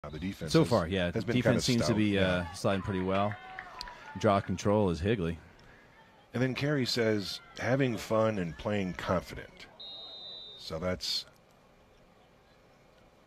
You Make the Call - Dangerous Play?
Did a player engage in actions that are rough, threatening, or without regard for player safety?

Note - Dangerous play may be carded.

2022 AZGLA Higley vs Pinnacle Varsity
Filmed by APN Sports